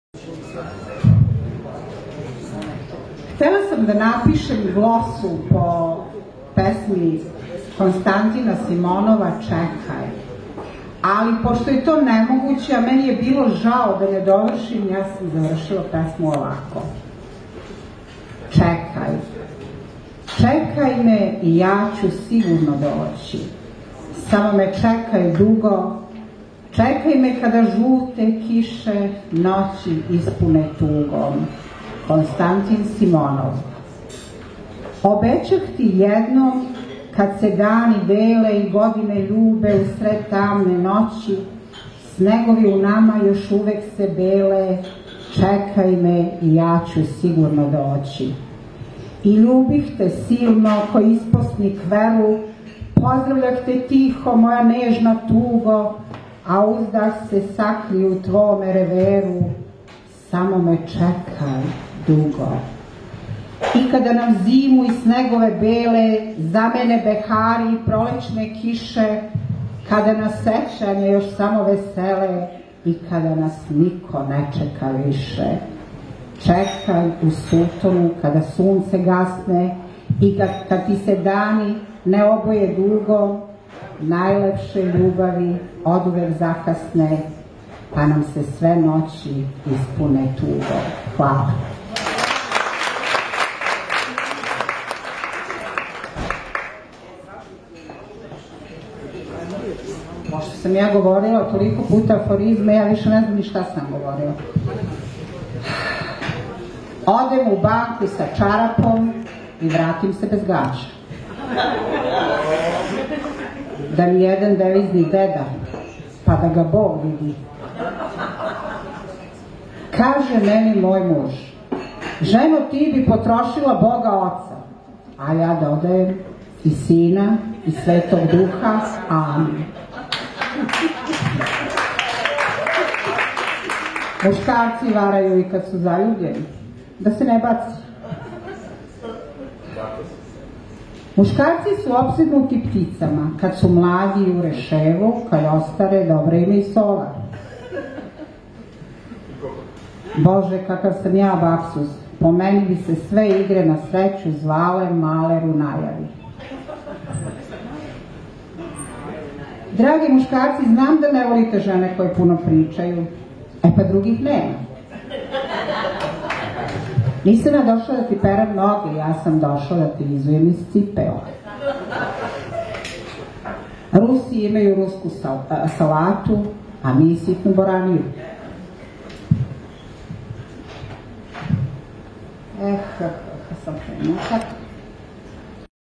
Магично вече 14 - Нада Караџић
Нада Караџић на Магичној вечери 14 у Улица мјузик бар. Балканска 13. Април 2019. Београд.